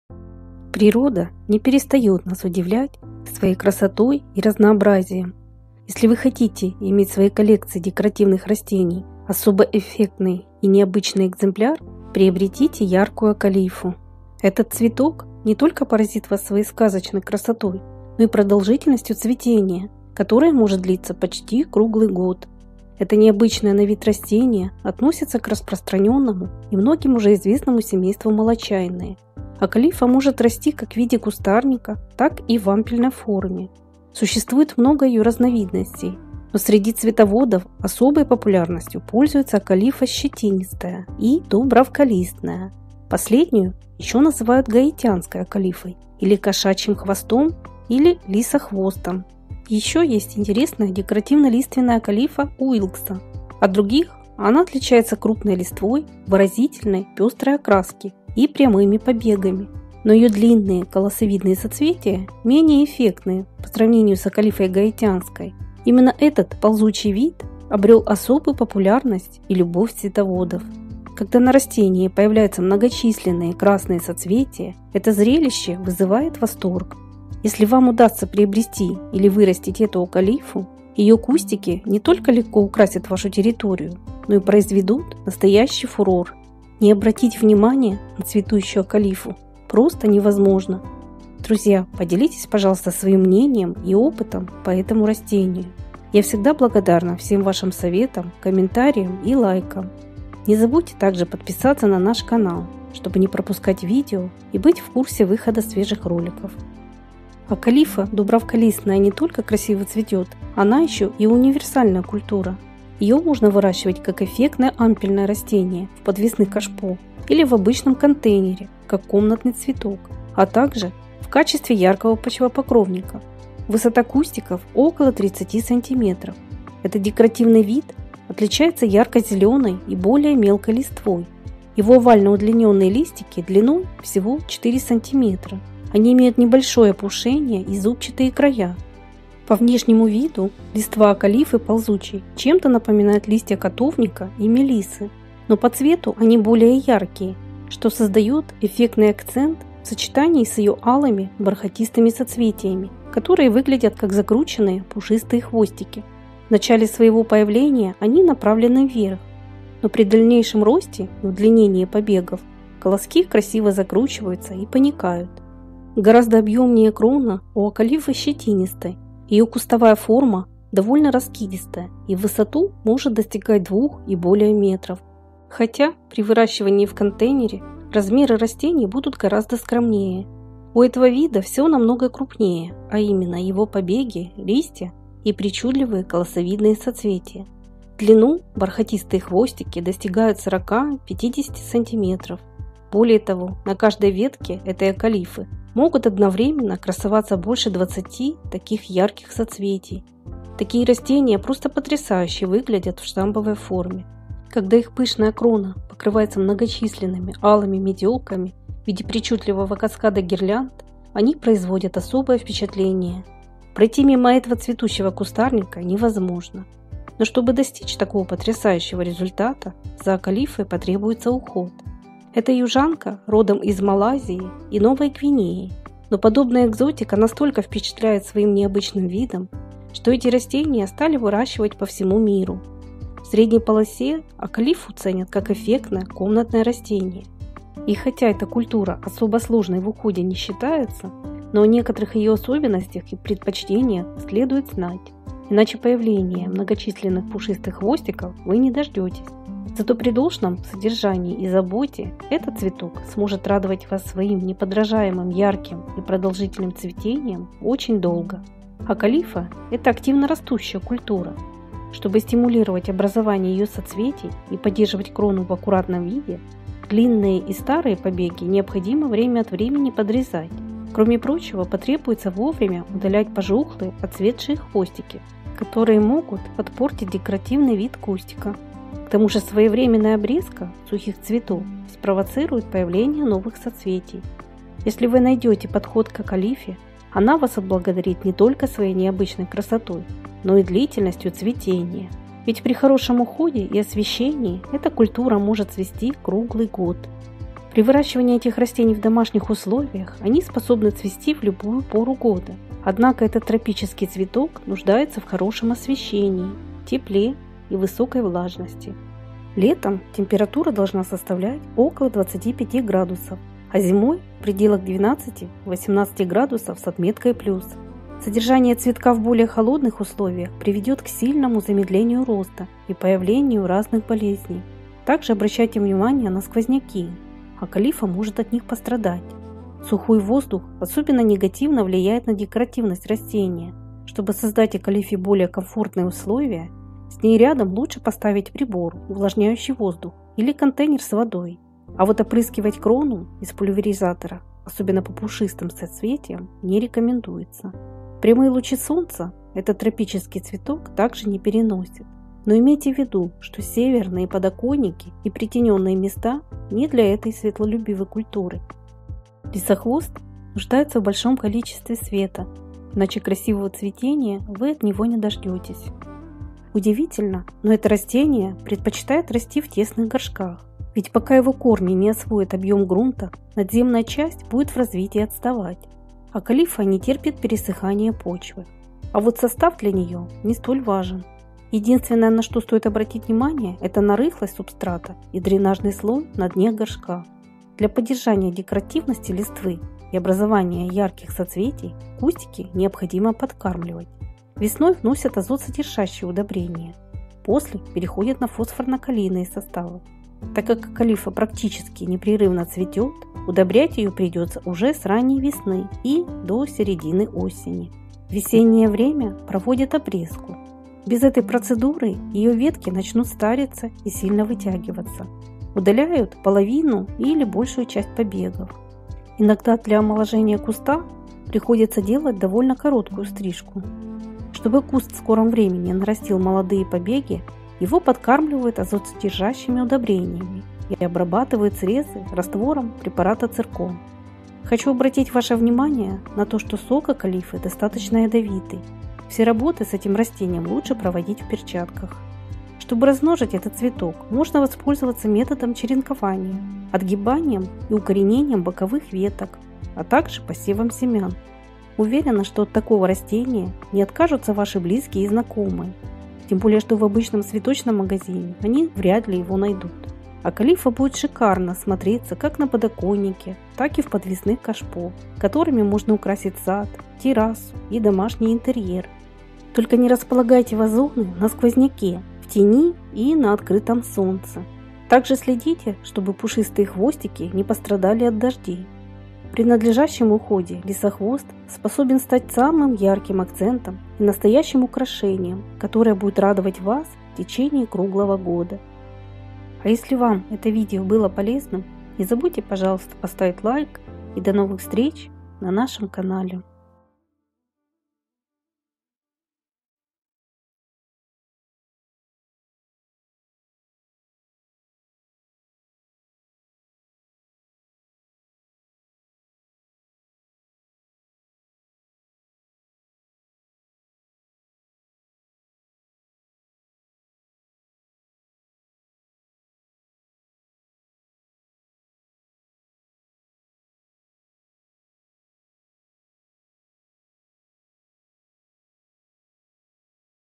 Восхитительно яркое цветение круглый год
Редкие растения в саду и дома.
Если вы хотите иметь в своей коллекции декоративных растений особо эффектный и необычный экземпляр, приобретите яркую Акалифу/Acalypha. Этот цветок не только поразит вас своей сказочной красотой, но и продолжительностью цветения, которое может длиться почти круглый год.
Это необычное на вид растение относится к распространенному и многим уже известному семейству Молочайные. Акалифа может расти как в виде кустарника, так и в ампельной форме. Существует много ее разновидностей. Но среди цветоводов особой популярностью пользуются акалифа щетинистая/Acalypha hispida и дубравколистная/Acalypha chamaedrifolia. Последнюю еще называют гаитянской акалифой или кошачьим хвостом, или лисохвостом.
Есть еще интересная декоративно-лиственная Акалифа Уилкса/Acalypha wilkesiana.
При выращивании этих растений в домашних условиях они способны цвести в любую пору года.
Акалифа будет шикарно смотреться как на подоконнике, так и в подвесных кашпо, которыми можно украсить сад, террасу и домашний интерьер. При надлежащем уходе лисохвост способен стать самым ярким акцентом и настоящим украшением, которое будет радовать вас в течении круглого года.